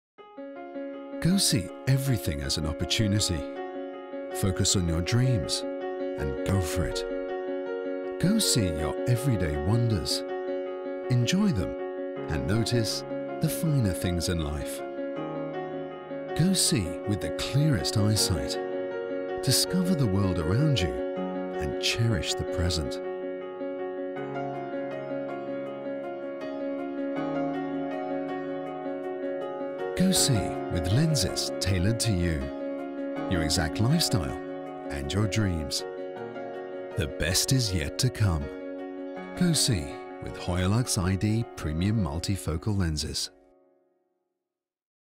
Go see with Hoyalux iD premium multifocal lenses
Enjoy your everyday wonders with Hoyalux iD premium multifocal lenses. The individualised lenses are designed to meet your specific needs, providing clear, natural sight at all distances, wide distortion-free fields of vision, superior image stability and a smooth transition between near and far.